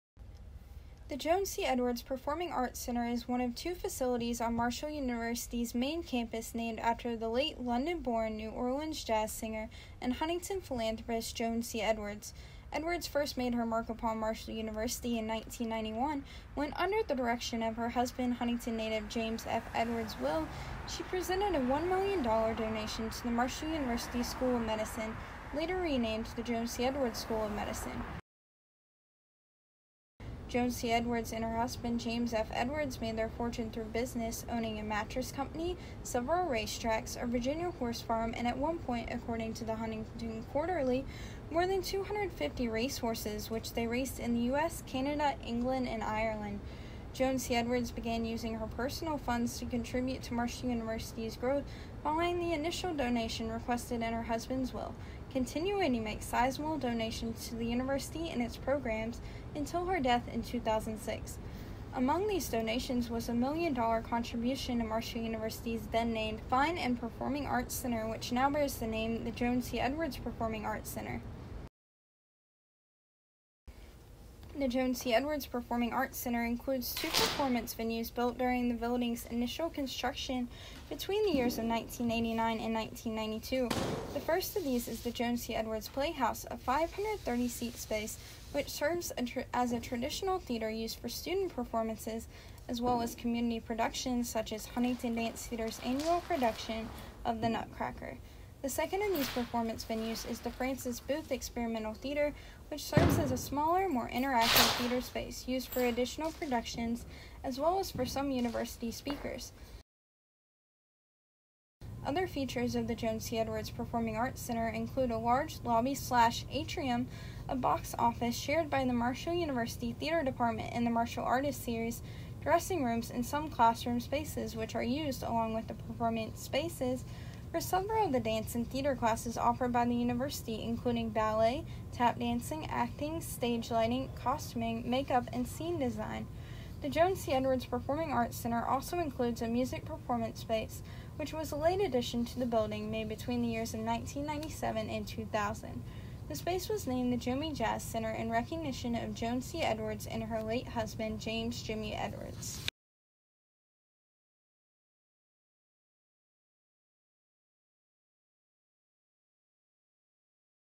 A Brief History of the Joan C. Edwards Performing Arts Center (360 Video)
This video is the final draft of a project that was completed for a Marshall University Journalism and Mass Communications digital imaging class. The video details the history of the Joan C. Edward Performing Arts Center, a performing arts venue in Huntington, West Virginia which bears the name of a London-born New Orleans jazz singer and Huntington philanthropist. The video, which was filmed in three locations within the building with the permission of its staff, was not created for the university's use or under its direction.